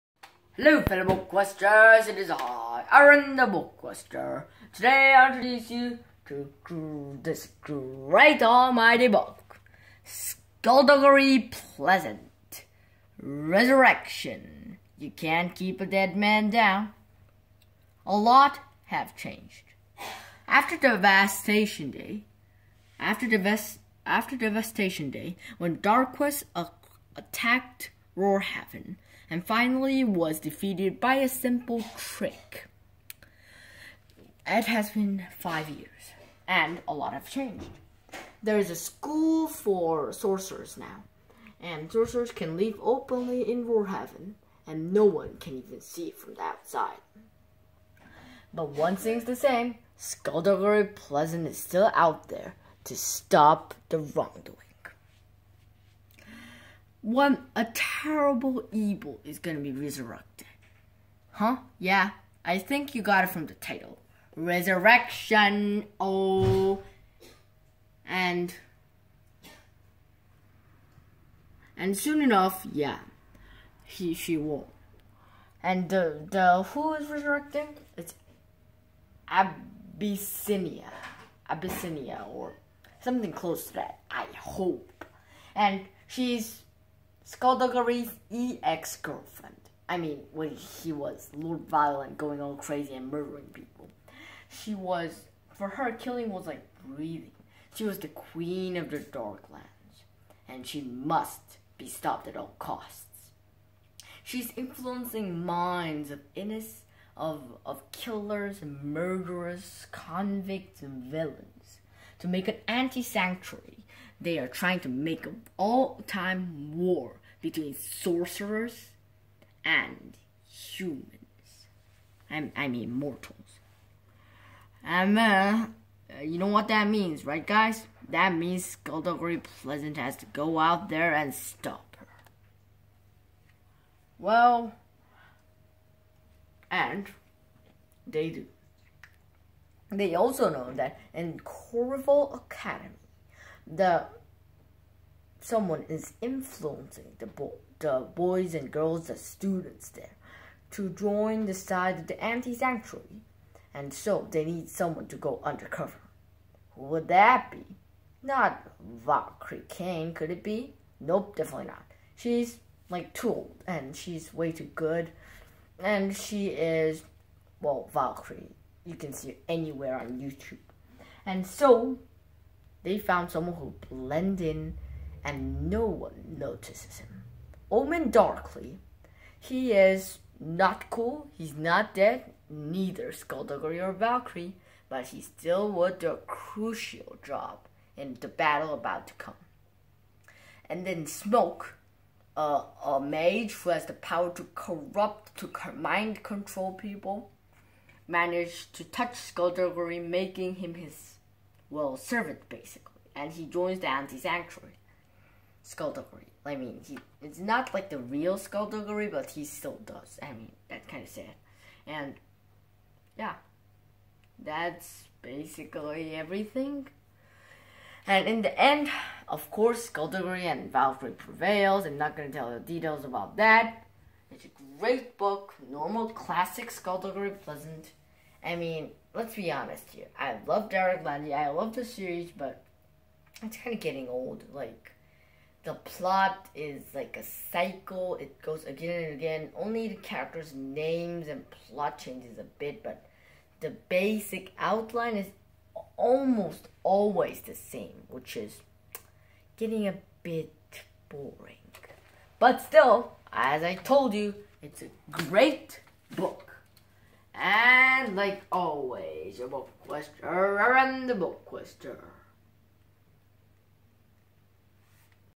[BookQuester] 'Skulduggery Pleasant 10 - Resurrection' (REVIEW)
- by Derek Landy
- Fantasy, Young Adult
- The skeleton detective is coming back to life...again!
- [영어책읽어주는 형아/ 북퀘스터]
- 해골 탐정
- 초등학생이 읽을만한 영어 소설, 어린이 영어 소설